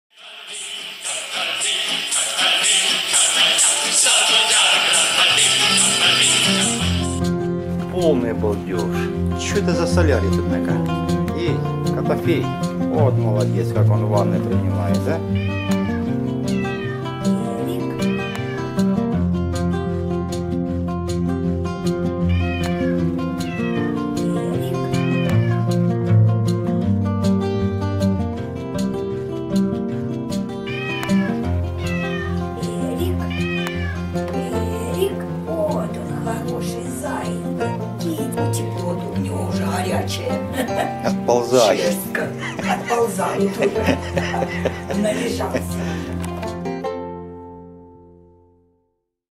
Уже март ,а коты не гуляют?.A cat warms up
Приметы на весну .Март, а коты не уходят в загул,значит...Видео-что коты делают в марте 2019г.